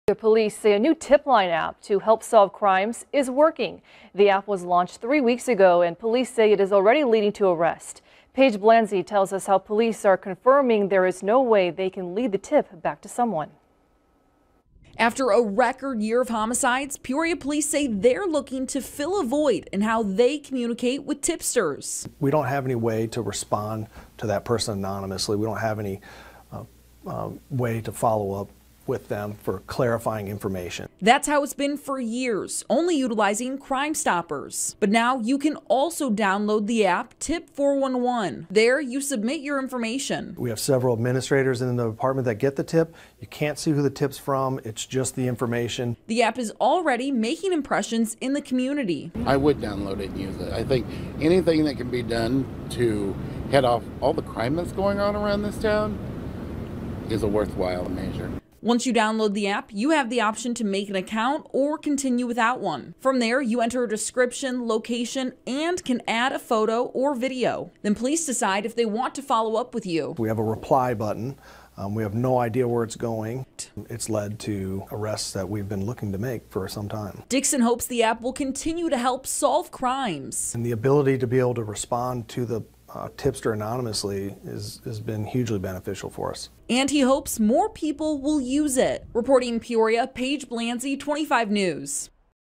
Peoria PD mobile app already leading to arrests
Courtesy 25 NEWS Peoria: Peoria, IL Police say their new Peoria PD mobile app, developed by tip411, is working to help solve crimes . The app was launched in January 2022 and police say it is already leading to arrests.